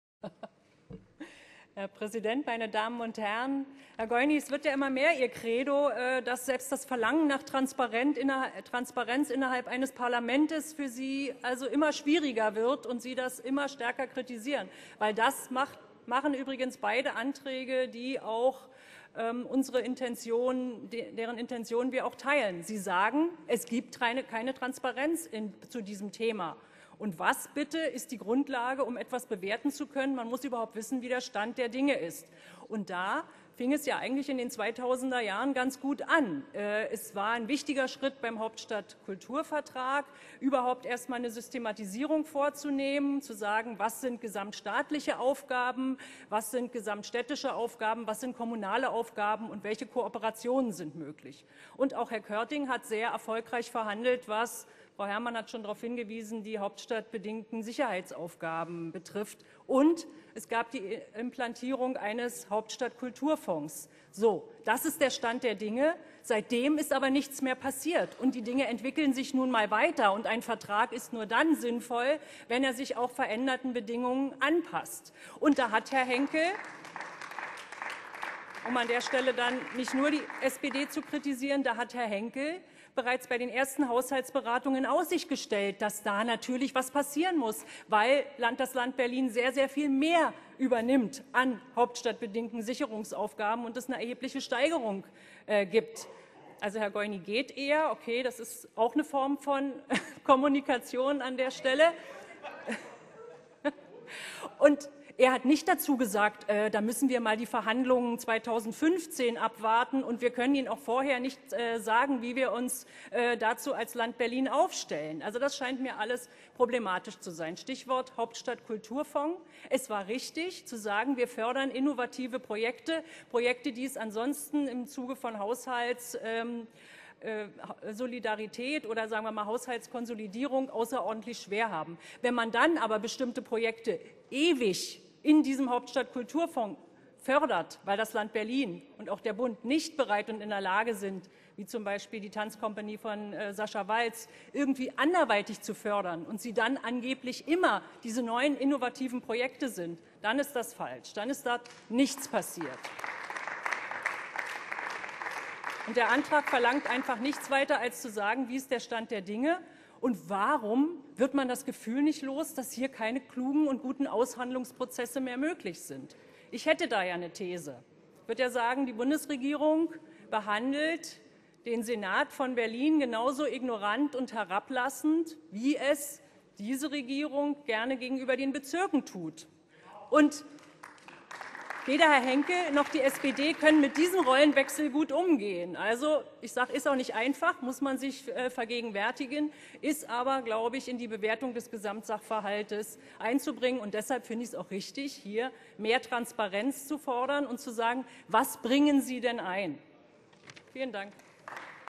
61. Sitzung - Carola Bluhm zu Neuverhandlung des Hauptstadtfinanzierungsvertrages
12. März 2015
61. Sitzung des Berliner Abgeordnetenhauses
 Carola Bluhm (Die Linke)

teilt die Intentionen der Anträge der Piratenfraktion zum Hauptstadtfinanzierungsvertrag. Es sei richtig, mehr Transparenz zu fordern. In den 2000er Jahren sei es ein wichtiger Schritt beim Hauptstadtkulturvertrag gewesen, eine Systematisierung vorzunehmen und zu sagen, was staatliche, städtische und kommunale Aufgaben seien. Seitdem sei aber nicht viel passiert, obwohl sich die Dinge weiter entwickelten. Ein Vertrag sei aber nur dann sinnvoll, wenn er sich veränderten Bedingungen anpasse.